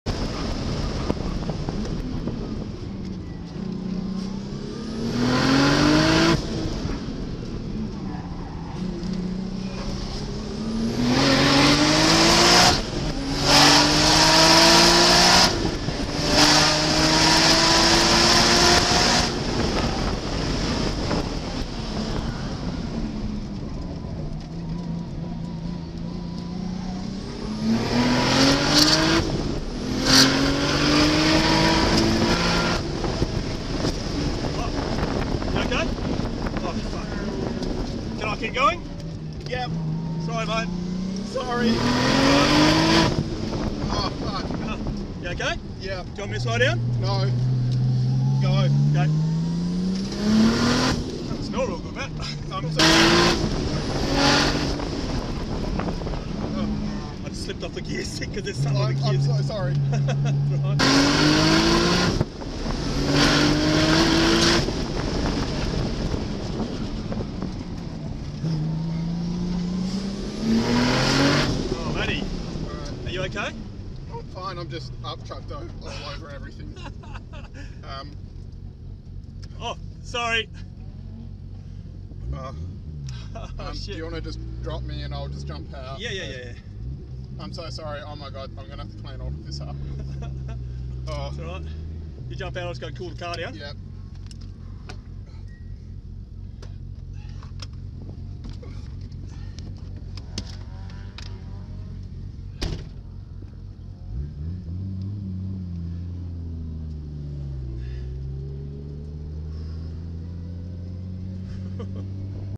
Shannon's Targa Rally Sprint - 2021 Season, Round 1 - Co-Driver Vomits
Well - sometimes things just happen.
Co-Driver had successfully been in the car for previous 3 runs of the course, but on this, out 4th run, it seems the Pulled Pork Taco he consumed earlier wanted out.  Kudos to him for continuing, this still ended up being the fastest run on the night and allowed us to take 1st place from a field of 80 cars.